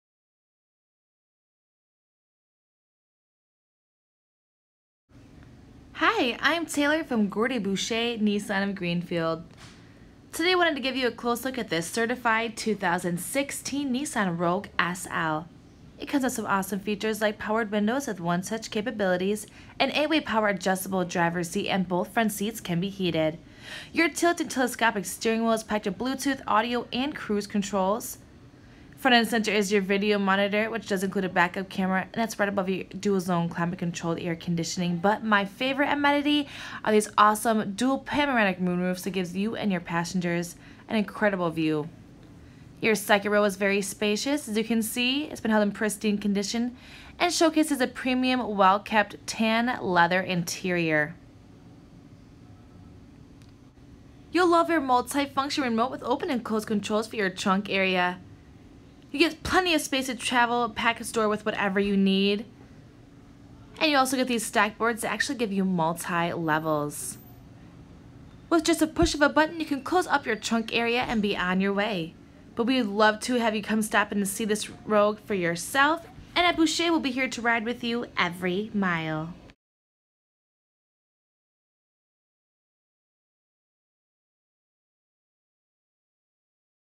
CERTIFIED 2016 NISSAN ROGUE SL- Gordie Boucher Nissan Of Greenfield
A quick video for one of our awesome customers here at Gordie Boucher Nissan Of Greenfield